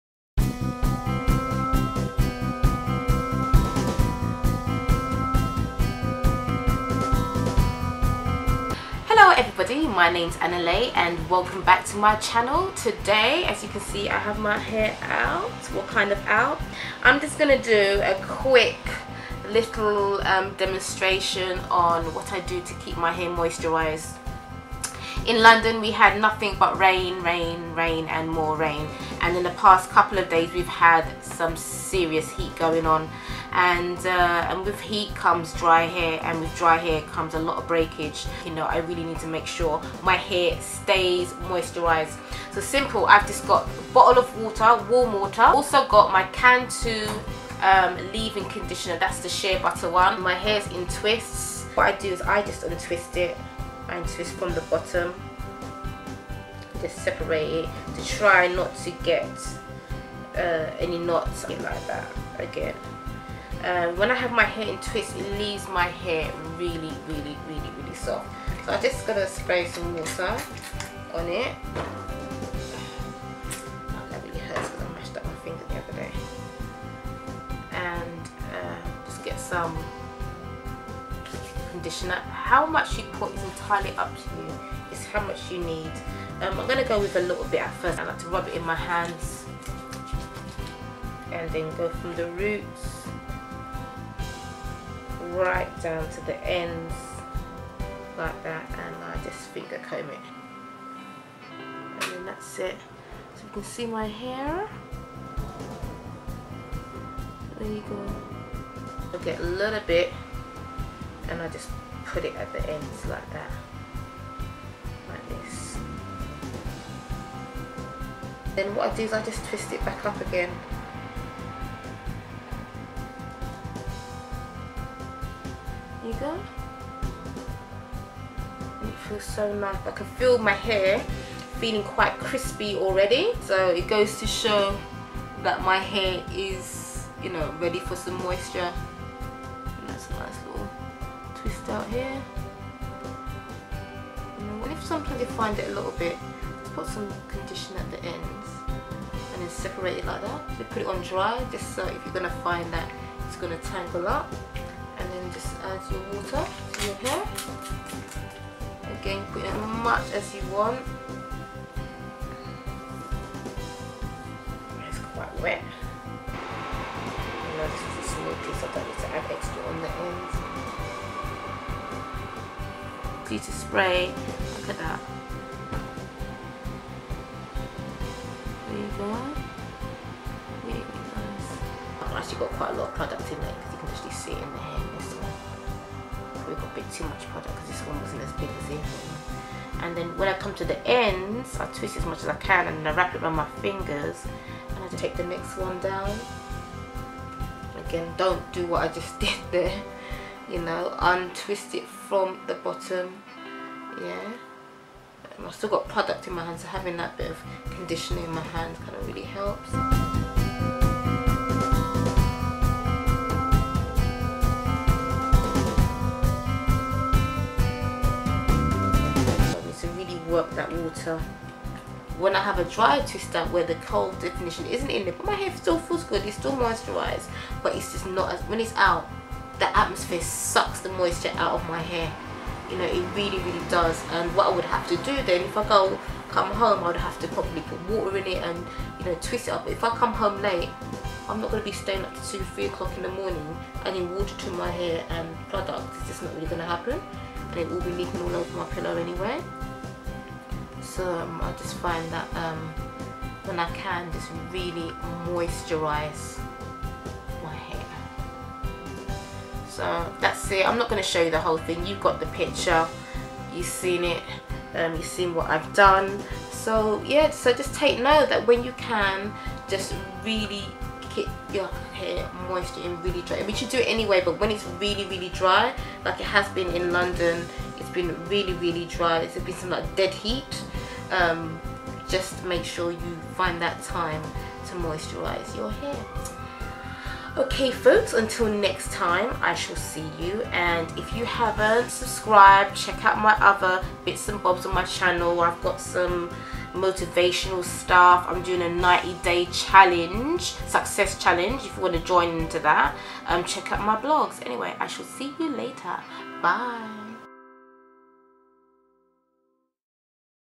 Moisturising natural hair with Cantu leave in conditioner
I like to use Cantu leave in conditioner as one of my staple hair products. This is a great way to maintain moisture.